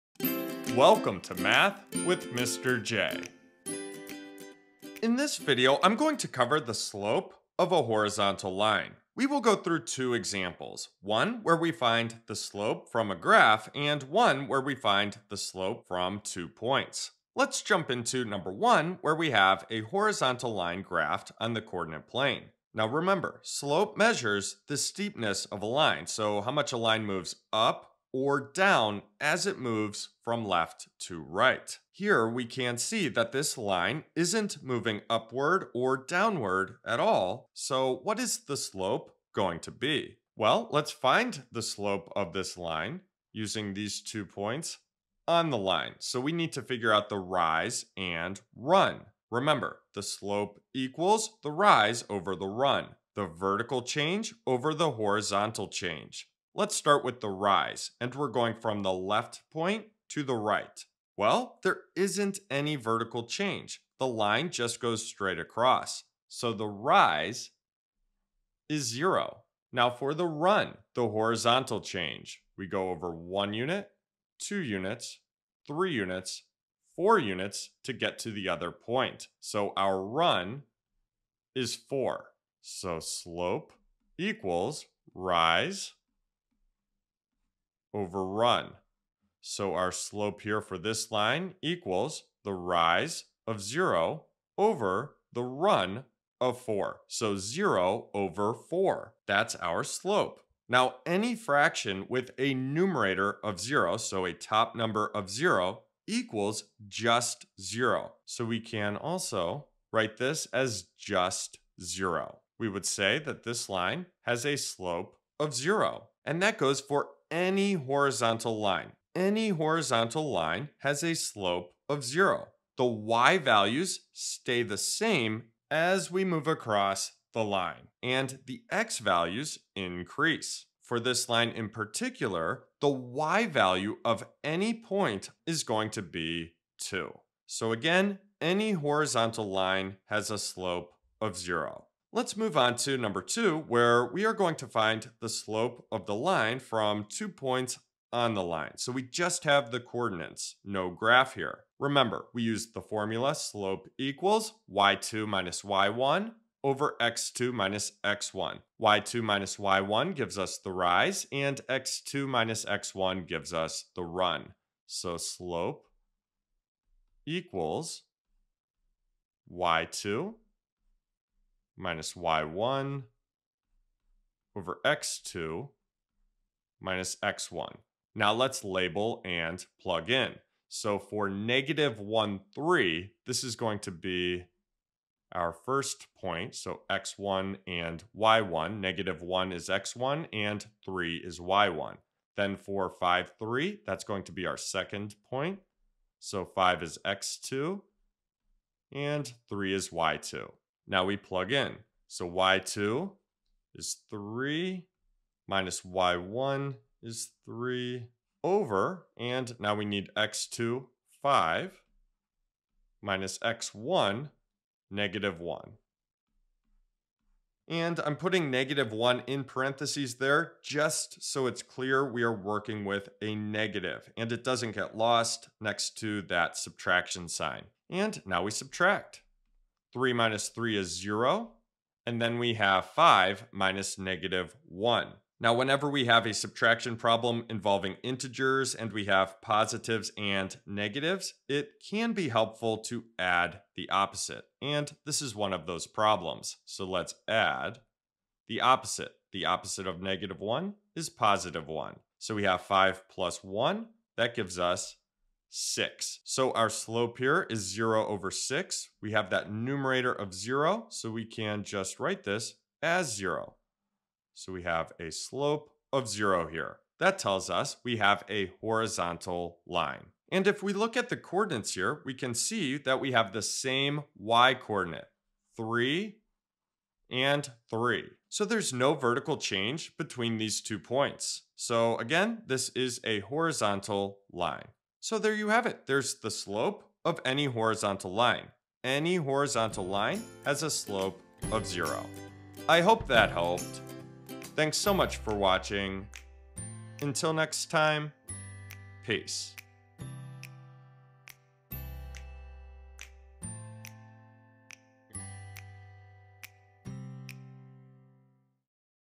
Slope of a Horizontal Line | Math with Mr. J
Welcome to Slope of a Horizontal Line with Mr. J! Need help with slope? You're in the right place!

Whether you're just starting out, or need a quick refresher, this is the video for you if you're looking for help with the slope of a horizontal line. Mr. J will go through two examples and explain what slope of any horizontal line is.

Hopefully this video is what you're looking for when it comes to slope.